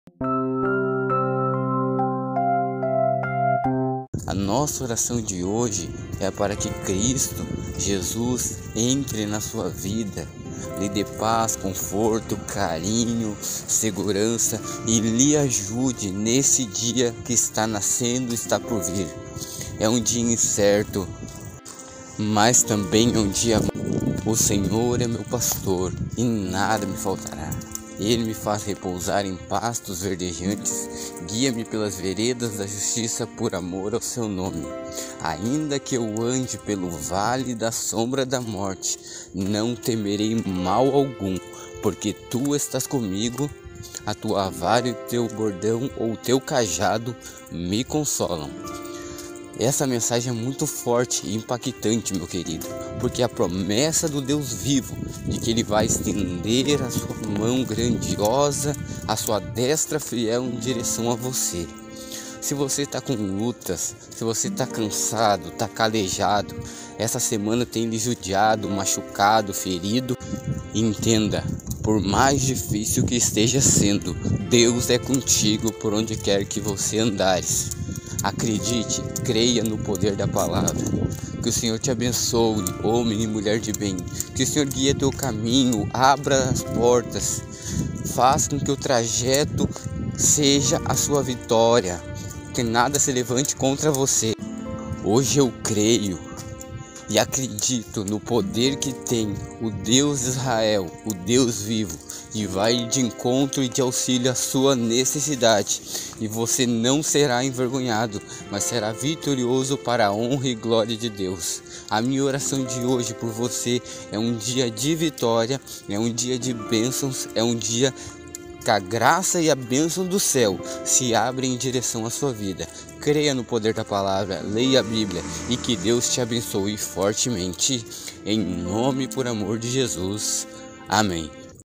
ORAÇÃO DO DIA ! 10/08/22 Oração da Bênção 🙏deixe seu pedido de oração
🙏 ORAÇÃO DO DIA | Deus tem uma benção para VOCÊ! 10 DE AGOSTO

oração do dia é um momento que você pode abrir seu coração e falar a Deus sobre seus pedidos e sentimos e clamar a Deus por seus cuidados.

a oração tem muito poder, como já diz o ditado muita oração muito poder!